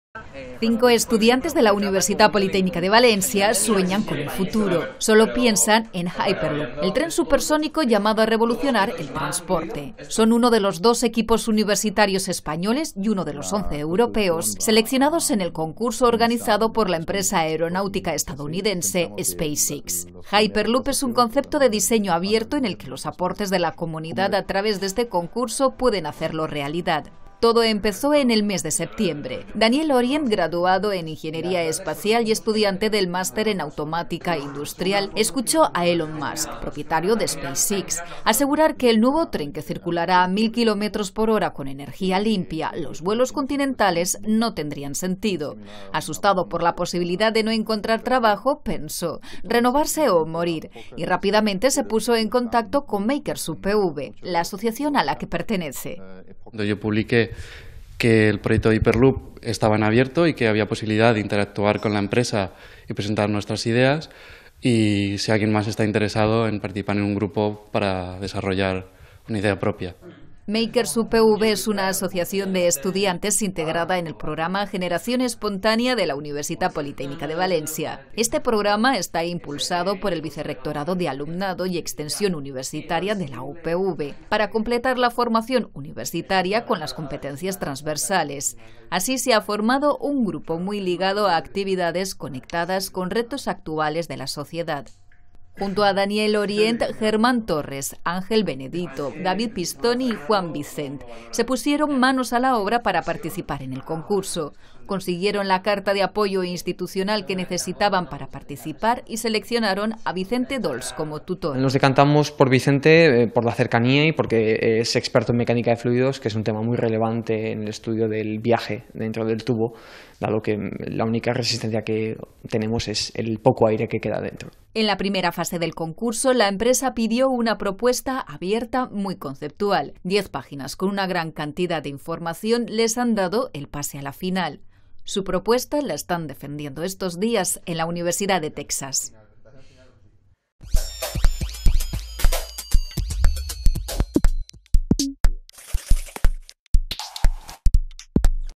Hyperloop UPV en Texas - Noticia @UPVTV, 29-01-2016
Cinco estudiantes de la Universitat Politècnica de València sueñan con el futuro. Sólo piensan en Hyperloop, el tren supersónico llamado a revolucionar el transporte. Son uno de los dos equipos universitarios españoles, y uno de los once  europeos, seleccionados en el concurso organizado por la empresa aeronáutica estadounidense SpaceX.

Hyperloop es un concepto de diseño abierto, en el que los aportes de la comunidad a través de este concurso pueden hacerlo realidad.

Todo empezó en el mes de septiembre. Daniel Orient, Graduado en Ingeniería Espacial y estudiante del Master en Automática Industrial en la UPV escuchó a Elon Musk, propietario de SpaceX, asegurar que, con el nuevo tren que circulará a 1.000 kilómetros por hora con energía limpia, los vuelos continentales no tendrían sentido. Asustado por la posibilidad de no encontrar trabajo pensó, renovarse o morir. Y, rápidamente se puso en contacto con Makers UPV, la asociación a la que pertenece.

Makers UPV es una asociación de estudiantes integrada en el programa Generación Espontánea de la Universitat Politècnica de Valencia. Este programa está impulsado por el vicerrectorado de Alumnado y Extensión Universitaria de la UPV para completar la formación universitaria con las competencias trasversales. Así se ha formado un grupo muy ligado a actividades conectadas con retos actuales de la sociedad.

Junto a Daniel Orient, Germán Torres, Ángel Benedito, David Pistoni y Juan Vicén se pusieron manos a la obra para participar en el concurso. Consiguieron la carta de apoyo institucional que necesitaban y seleccionaron a Vicente Dolz como tutor.

En la primera fase del concurso la empresa pidió una propuesta abierta muy conceptual. Diez páginas con una gran cantidad de información les han dado el pase a la final.

Su propuesta la están defendiendo estos días en la Universidad de Texas.